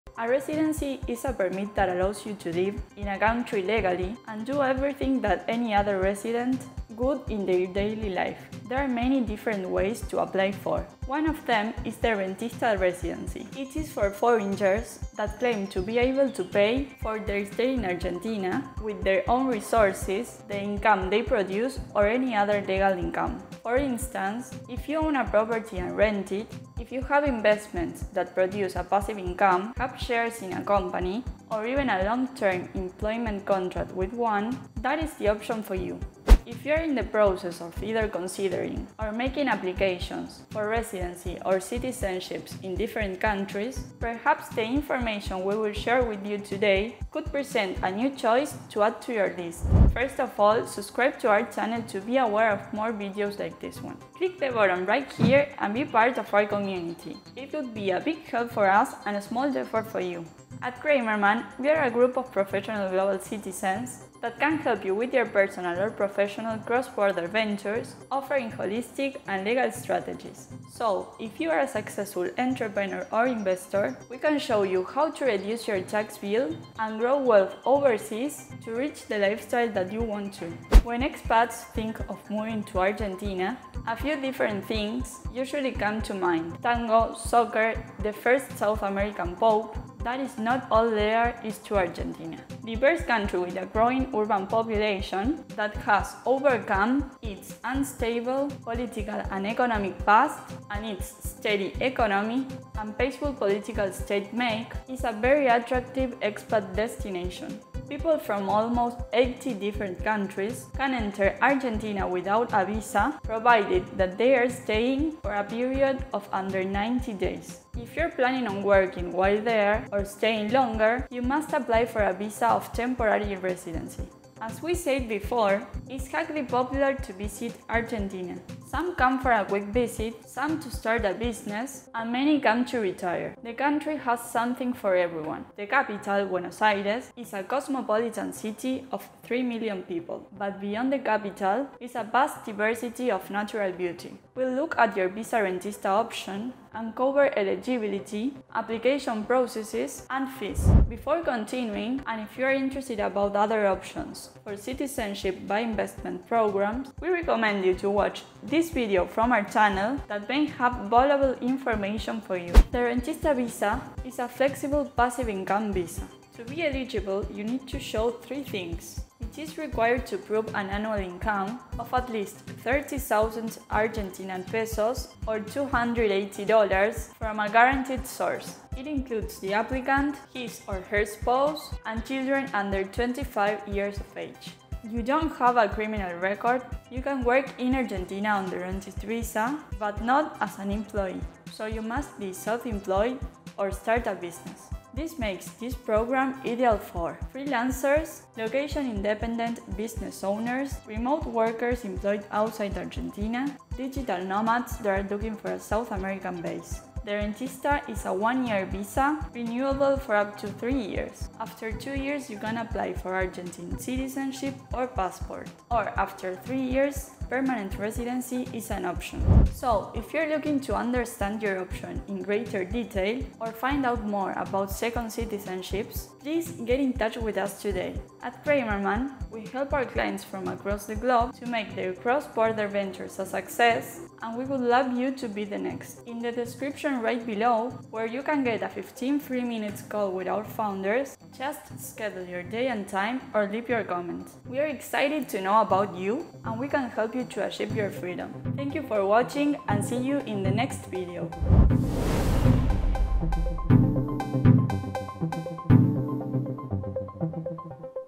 Rentist Visa in Argentina | All You Have To Know | Permanent Residency in Argentina | Digital Nomads
A residency is a permit that allows you to live in a country legally and do everything that any other resident would in their daily life.

There are many different ways to apply for: One of them is the Rentista Residency and it is for foreigners that claim to be able to pay for their stay in Argentina  with their own resources, the income they produce or any other legal income.